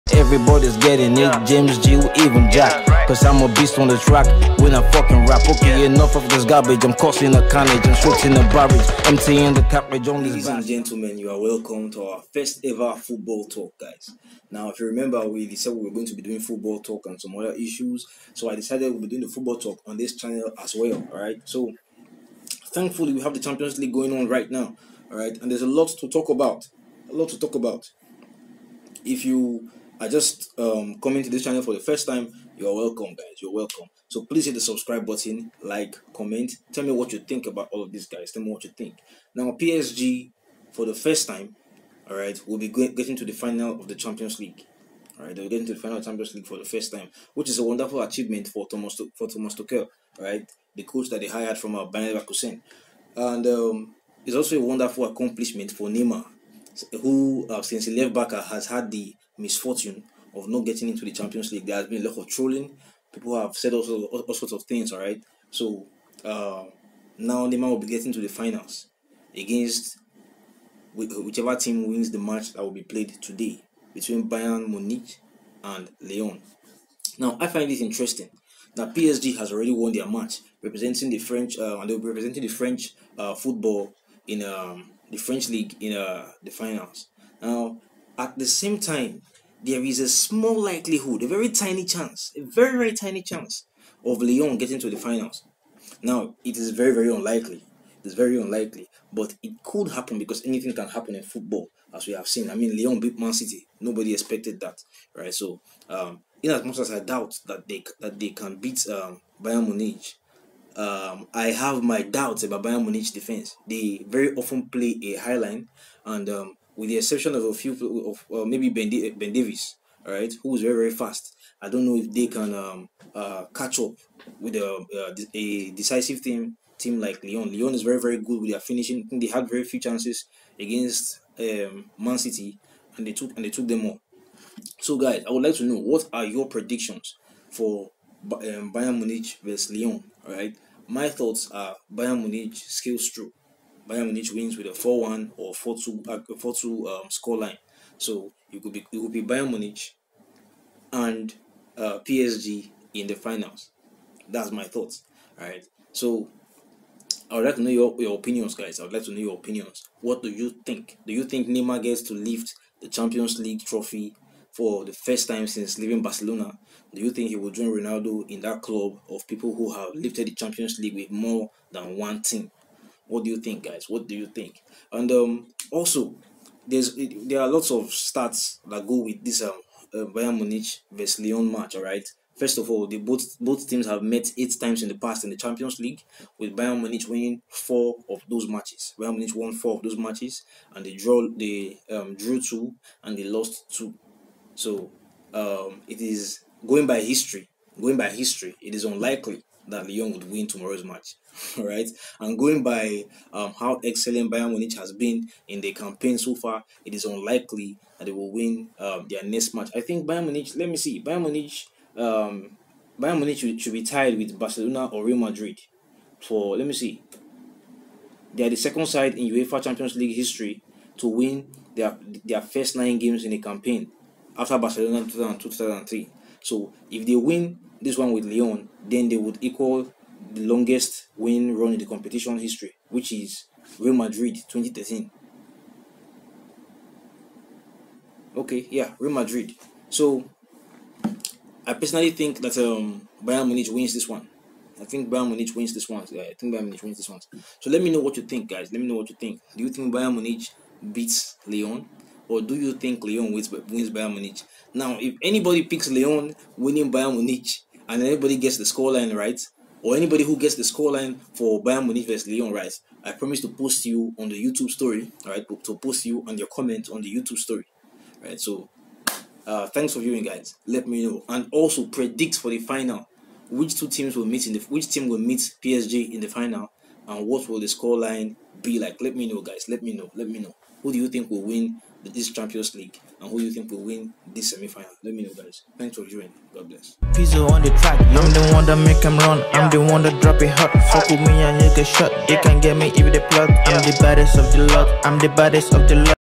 Neymar and PSG make it to the final; Bayern v Lyon- A French Final? Our first Football talk
Neymar, Mbappe and Di Maria lead Thomas Tuchel's PSG to the club's first UEFA Champions League final. Will Bayern meet them on the D-day? Or will Lyon pull another upset to make the D-day be an all-French affair?

What are your opinions, guys?